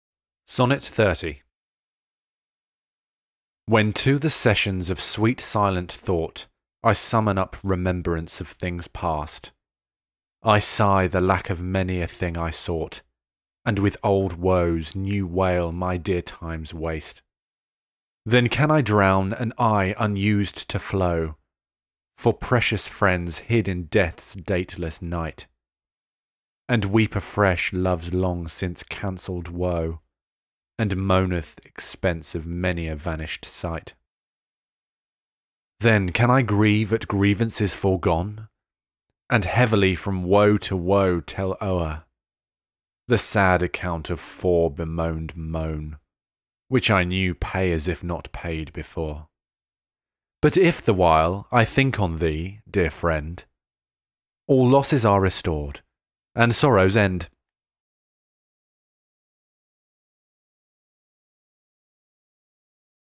Sonnet 30: When to the sessions of sweet silent thought
Sonnet 30 by Shakespeare; read by Jamie Muffett.